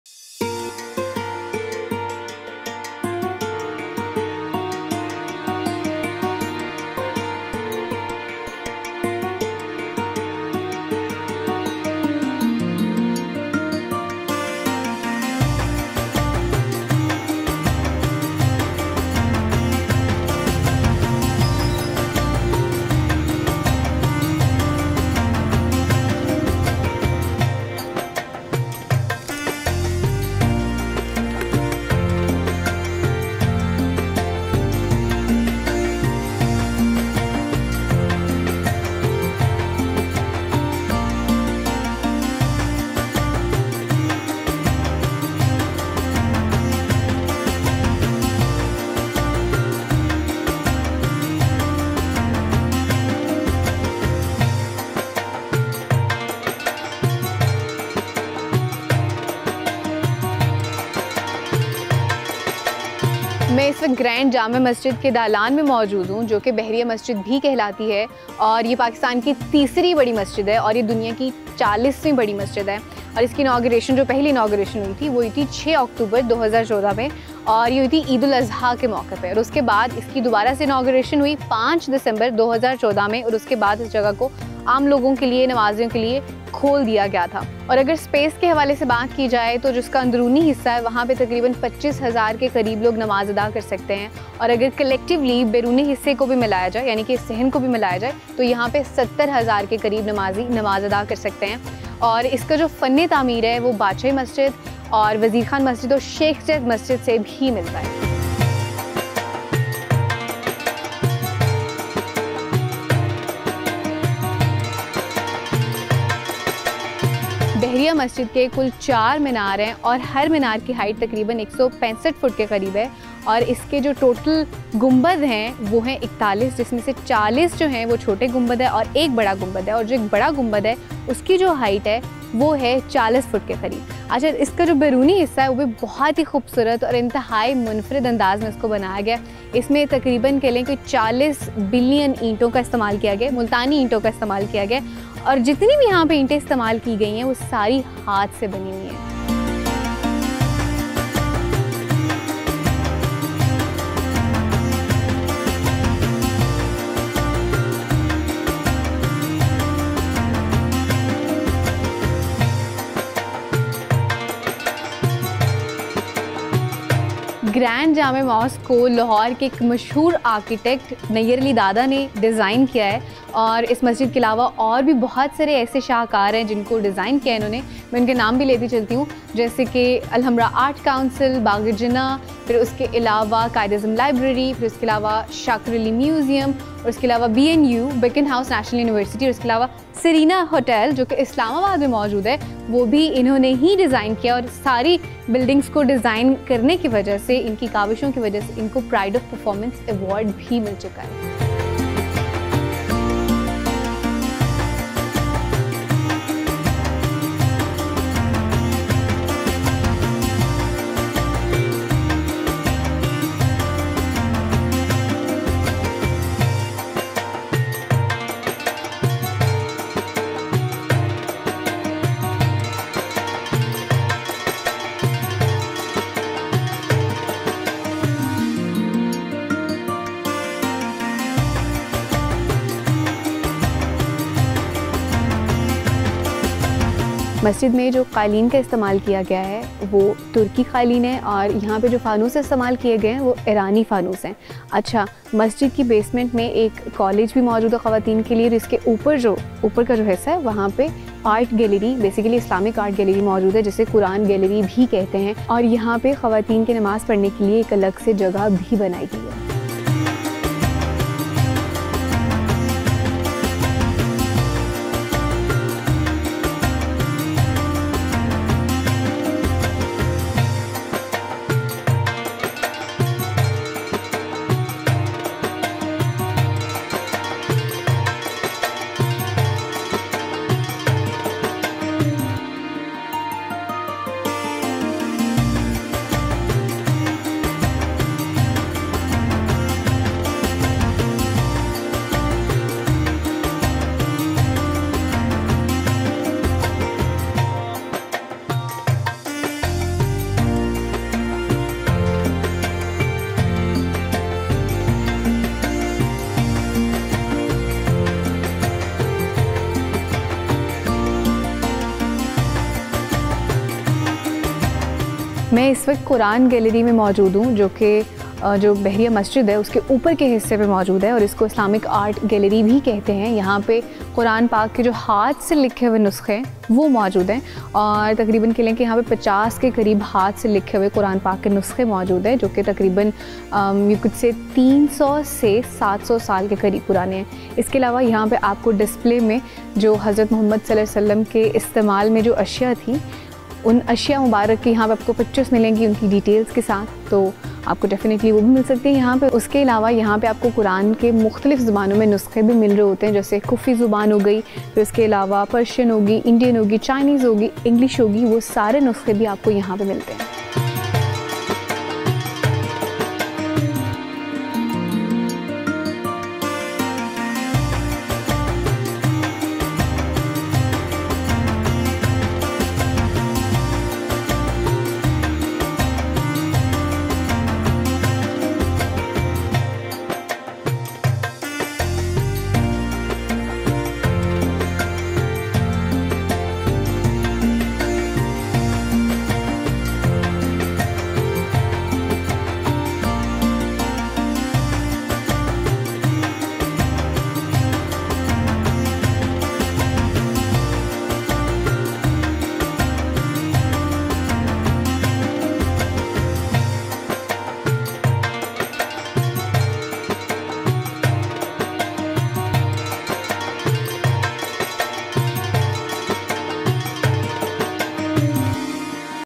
Inside Grand Jamia Masjid Bahria Town Lahore - You Really Don't Want to Miss This
Grand Jamia Mosque Lahore is located in Bahria Town, Lahore, Pakistan. With a capacity of 70,000 worshippers, it is the third-largest mosque in Pakistan and the fourteenth-largest mosque worldwide.